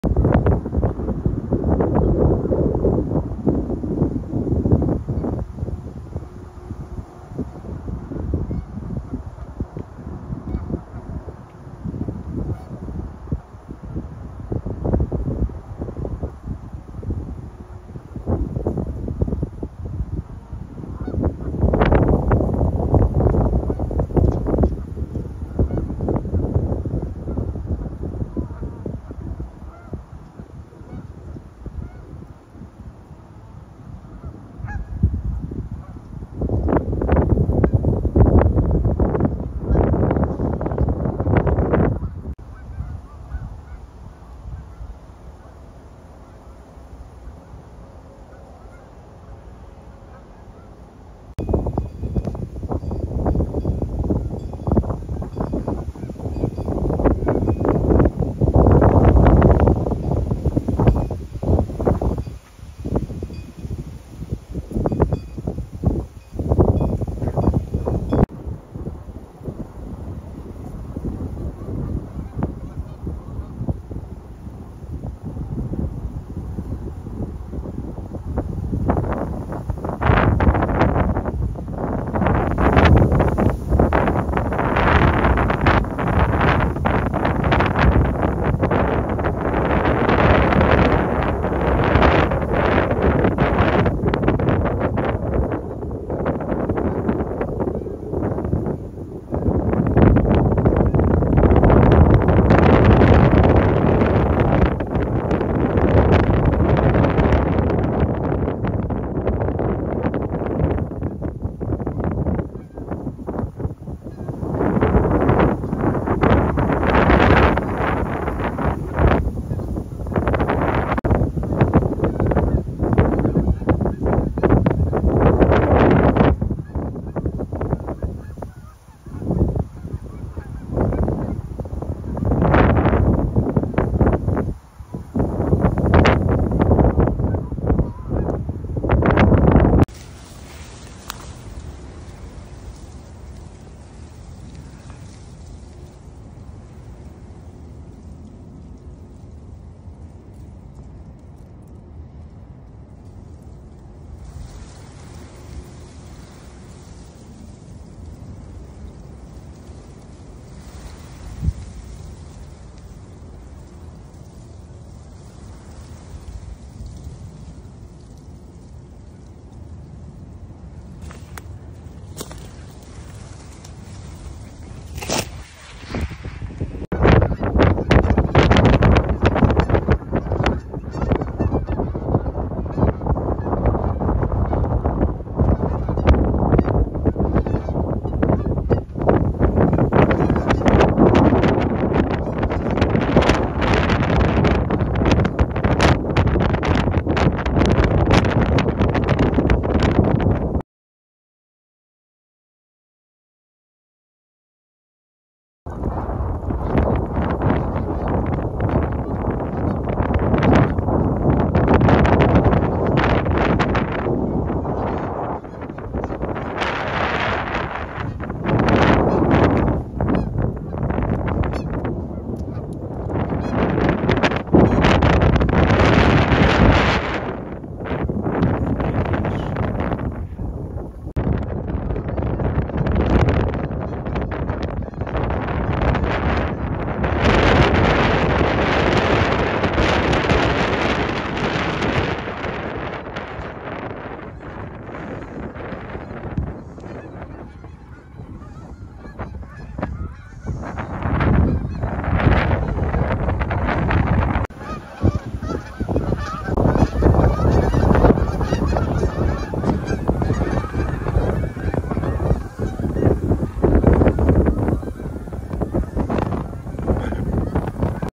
Whipping Winds/low flying geese
Some overlooking, patrolling the park. Wondering what’s in store. Lots of wind noise, and honking geese, as they scatter for cover, plus trying to avoid me. It was an enjoyable mile hike. nature naturelovers getoutdoors.